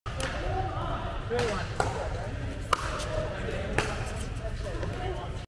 Last chance: Watch the Serve to be BANNED in 2023
Pickleball Spin serve will be banned in 2023
to find out why